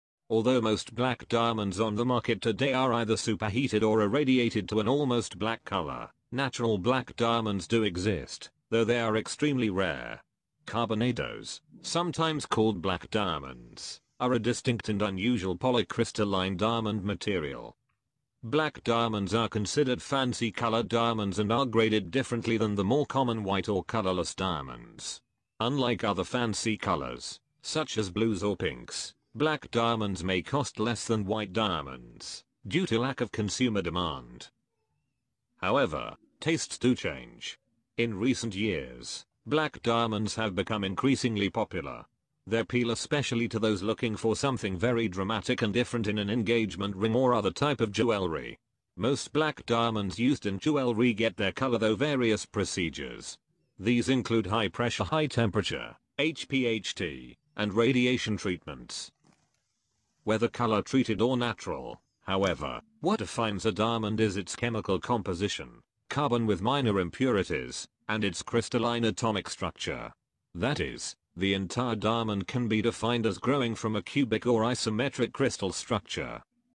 Black diamond | carbonado |test and information rough diamonds
Although most black diamonds on the market today are either superheated or irradiated to an almost black color, natural black diamonds do exist, though they are extremely rare. Carbonados, sometimes called "black diamonds," are a distinct and unusual polycrystalline diamond material.black diamond equipmentenigma black diamondfaceted black diamondHow to check black diamondtreated black diamondtreated Black diamond